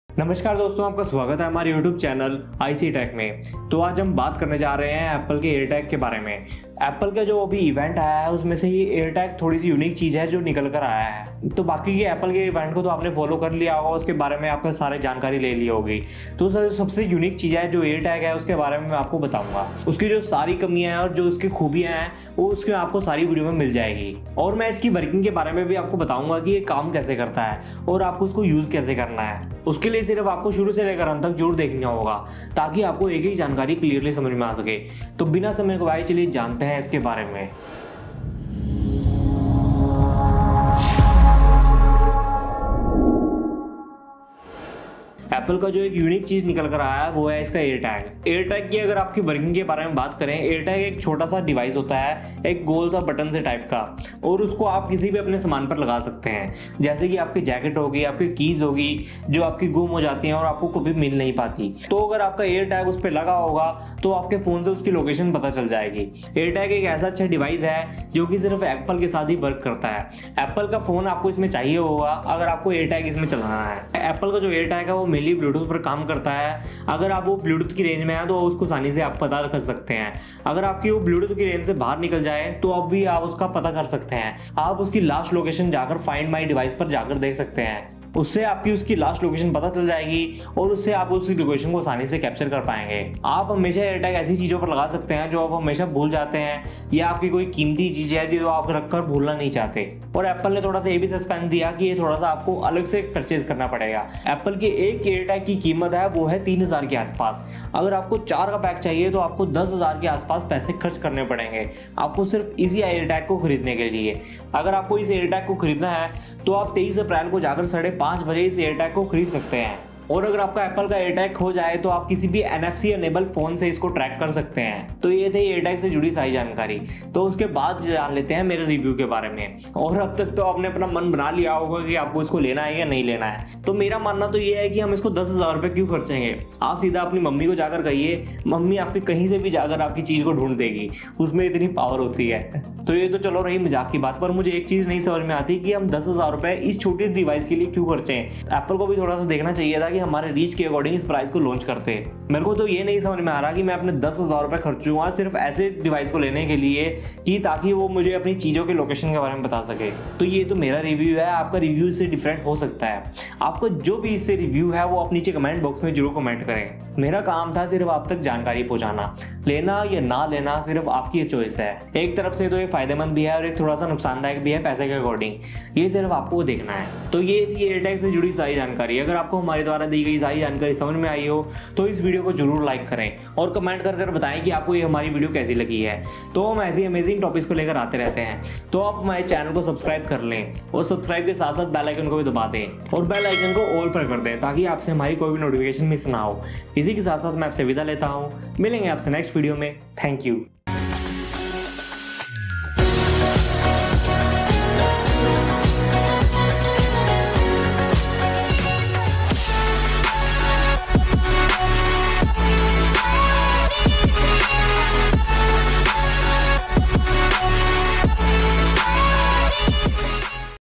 Is It is Worth To Buy Apple's New AirTag ? 2021 | Honest Reviews ‎️‍🔥‎️‍🔥
(हिंदी) What is AirTag: All details about the Apple AirTag .All your top questions answered about Apple’s Bluetooth tracker. I Also Give My Honest Reviews About Apple Airtag . All Of Your Questions clear in this video like price ,security,working,and many more of your Doubts

Tripod Used
Mic Used
Phone Camera Used

ICA TECH
(IT'S CONFIDENTIAL about Tech)

Keep visiting for more knowledge topics